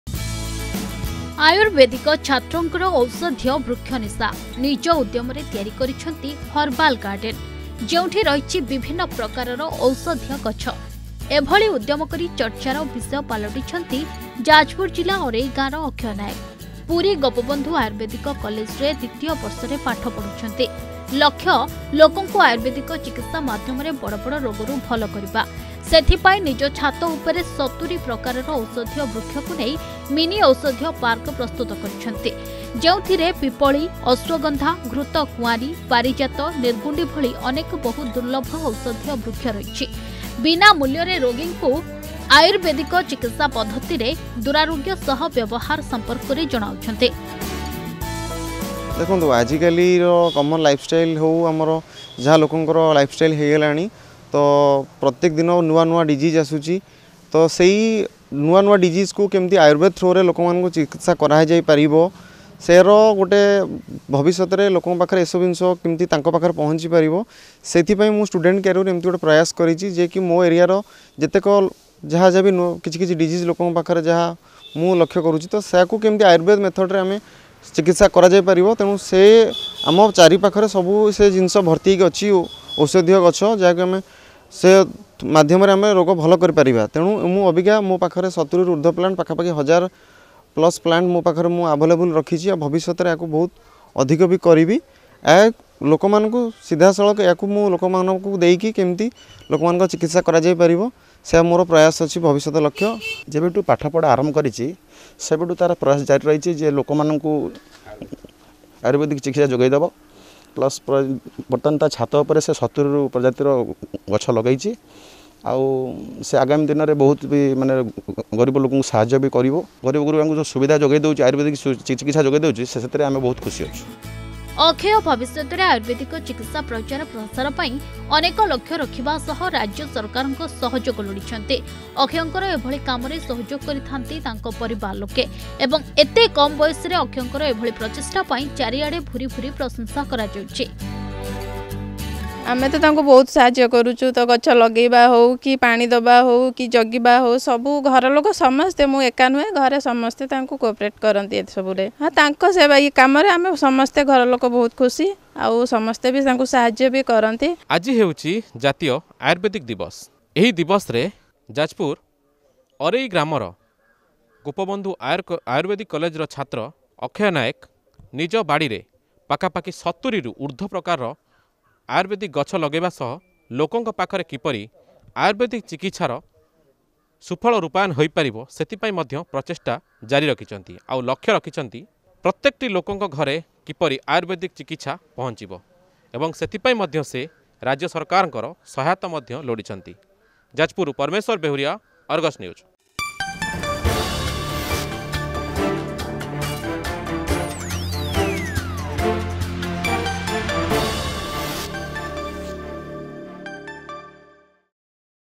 ଆୟୁର୍ବେଦିକ ଛାତ୍ରଙ୍କ ଔଷଧୀୟ ବୃକ୍ଷ ନିଶା
ଆଜି ହେଉଛି ଜାତୀୟ ଆୟୁର୍ବେଦିକ ଦିବସ । ହଜାର ହଜାର ବର୍ଷ ପୂର୍ବରୁ ଆୟୁର୍ବେଦିକର ଚିକିତ୍ସା ପଦ୍ଧତି ଲୋକଙ୍କୁ ବହୁ ଦୂରାରୋଗ୍ୟରୁ ଆରୋଗ୍ୟ କରାଉଥିଲା । ହେଲେ ଆଜିର ଦିନରେ ଏଲୋପାଥି ଔଷଧର ବହୁଳ ବ୍ୟବହାର ଭିତରେ ଲୁଚି ଯାଉଛି ଆୟୁର୍ବେଦିକ ଚିକିତ୍ସା ପଦ୍ଧତି ...
ଅଧିକ ଖବର ପାଇଁ ଡ଼ାଉନଲୋଡ କରନ୍ତୁ “Argus News” ଆପ୍